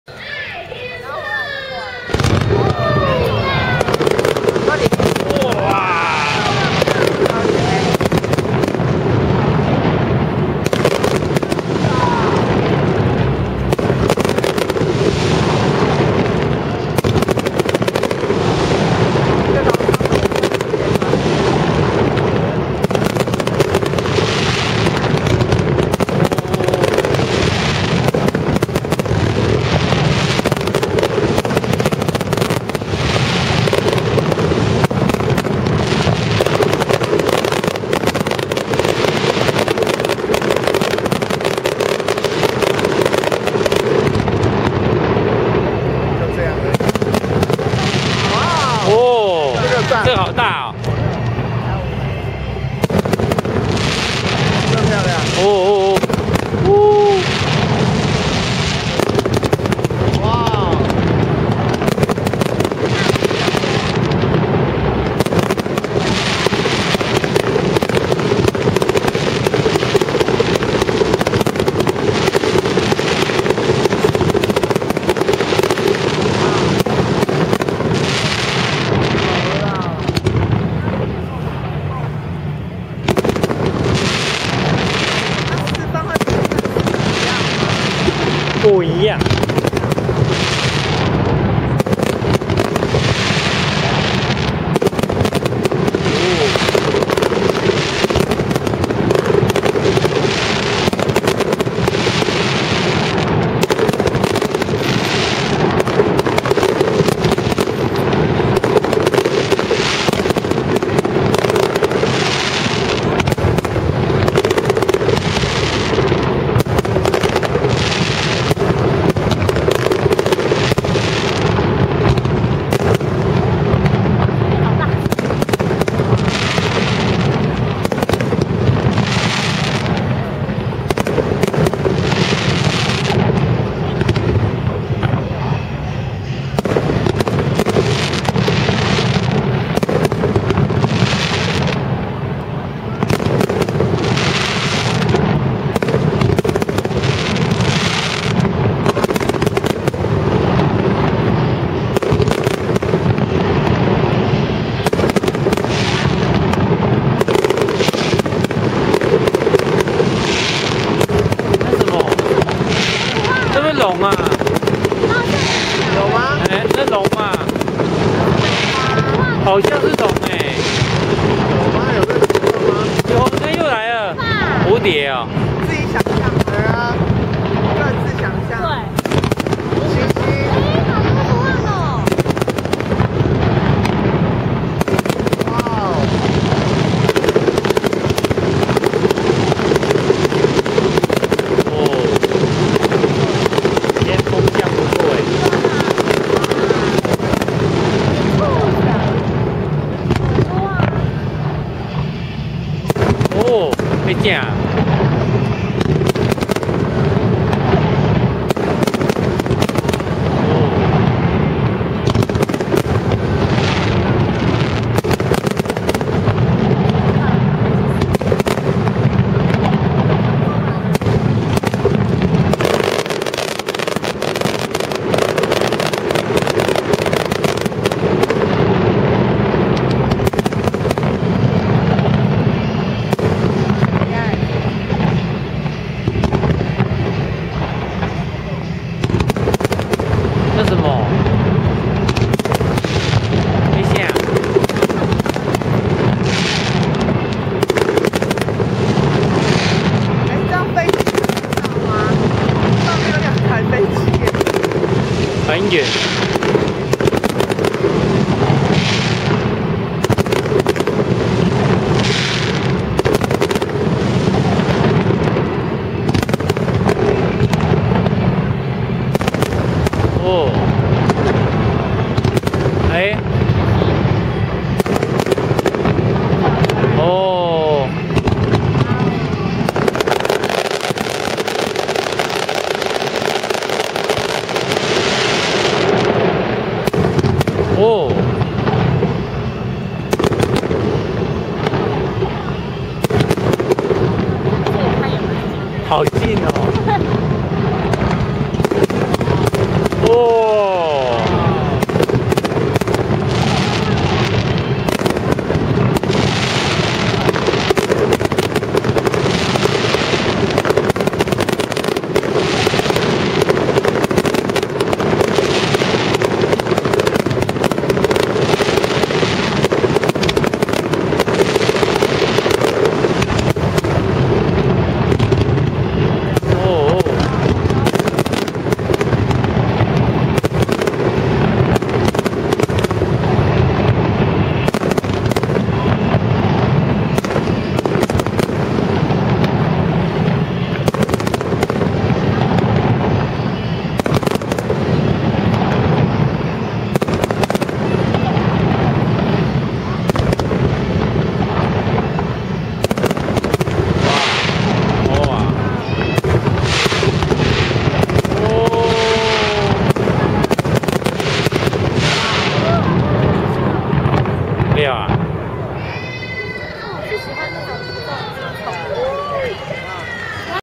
2012-01-27 高雄燈會藝術節 Kaohsiung Lantern Festival 煙火試放總彩排
今天在光榮碼頭試放煙火最後一次彩排，禮拜天1/29就正式開幕煙火秀囉！